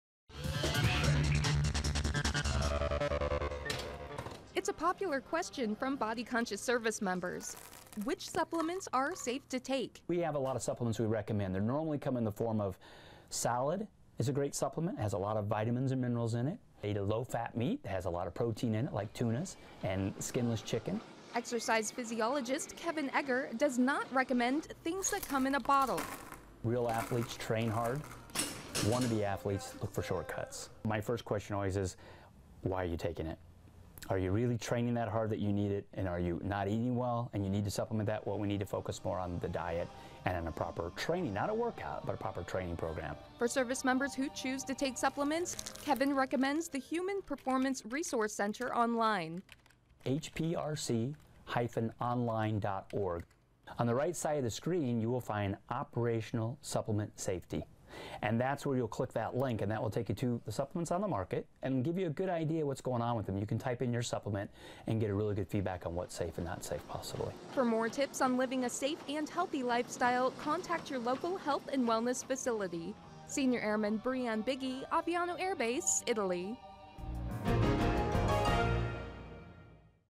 MilitaryPTFitness.com - Air Force Report: Supplement Safety
- Supplements can help or harm your health depending on how many or which ones you take. Senior Airman Breanne Biggie has some tips on supplement use in the military. Also available in high definition